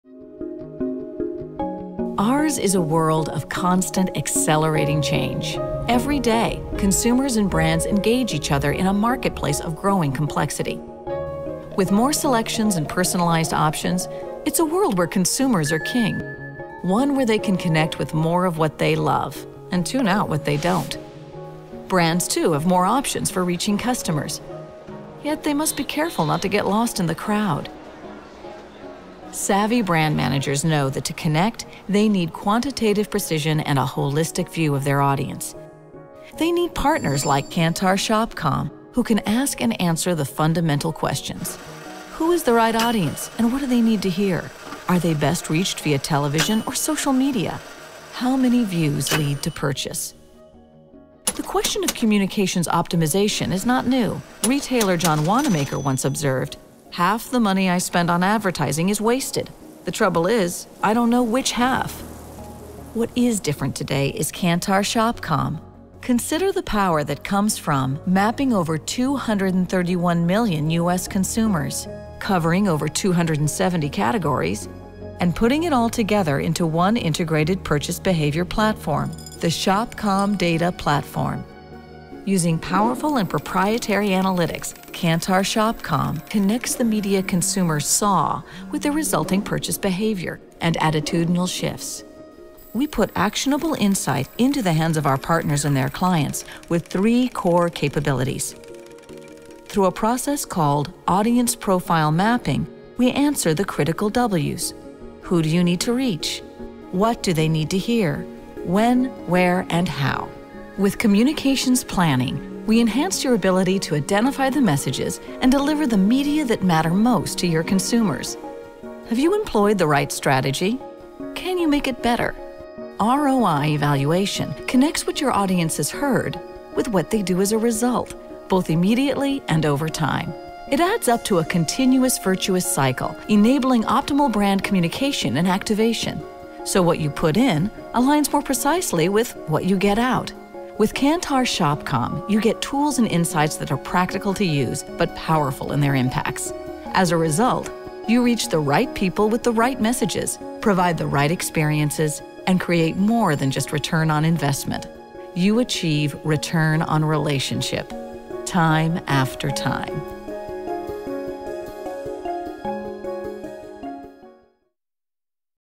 Kantar Shopcom - Brand Building in the Age of Perpetual Connectedness
Ours is a world of constant, accelerating change. Every day, consumers and brands engage each other in a marketplace of growing complexity.

With more selections and personalized options...it's a world where consumers are king. One where they can connect with more of what they love -- and tune out what they don't.

Brands too have more options for reaching customers. Yet, they must be careful not to get lost in the crowd.

Savvy brand managers know that to connect, they need quantitative precision and a holistic view of their audience. They need partners like Kantar Shopcom who can ask and answer the fundamental questions.

Who is the right audience and what do they need to hear? Are they best reached via television or social media? How many views lead to purchase?

The question of communications optimization is not new. Retailer John Wannamaker once observed: "Half the money I spend on advertising is wasted; The trouble is, I don't know which half."

What is different today is Kantar Shopcom. Consider the power that comes from:
-- Mapping over 231 million US consumers.
-- Covering over 270 categories.
-- And putting it all together into one integrated purchase behavior platform...The Shopcom Data Platform™.

Using powerful and proprietary analytics, Kantar Shopcom connects the media consumers saw with their resulting purchase behavior and attitudinal shifts. We put actionable insight into the hands of our partners and their clients with three core capabilities:

Through a process called Audience Profile Mapping™, we answer the critical "W"s -- WHO do you need to reach? WHAT do they need to hear? WHEN, WHERE and HOW?

With Communications Planning, we enhance your ability to identify the messages and deliver the media that matter most to your consumers.

Have you employed the right strategy? Can you make it better? ROI Evaluation connects what your audience has heard with what they do as a result -- both immediately and over time.

It adds up to a continuous virtuous cycle -- enabling optimal brand communication and activation. So, what you put in aligns more precisely with what you get out.

With Kantar Shopcom, you get tools and insights that are practical to use but powerful in their impacts.

As a result... you reach the right people with the right messages...provide the right experiences...and create more than just return on investment. You achieve return on relationship. Time after time.